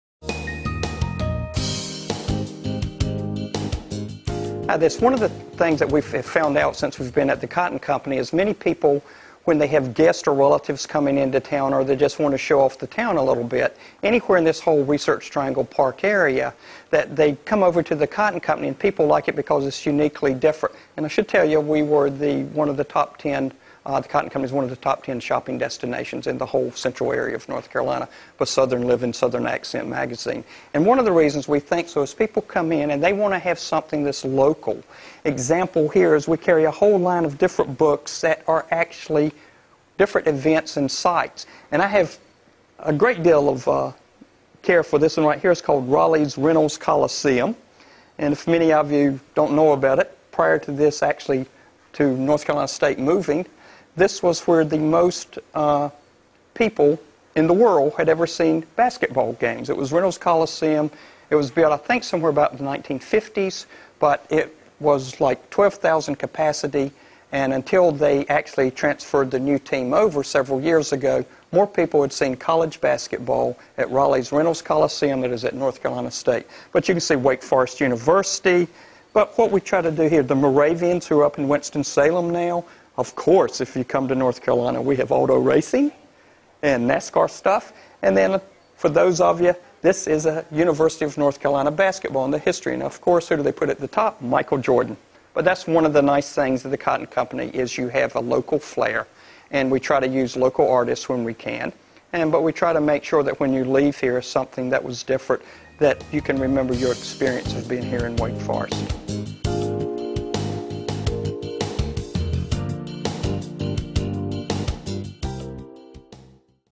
The Cotton Company Offers Books Of Local Interest
local books for souvenir gifts of N.C., Wake Forest, UNC, Duke and other historical gifts offered at The Cotton Company, the one stop gift shop for all gift giving events.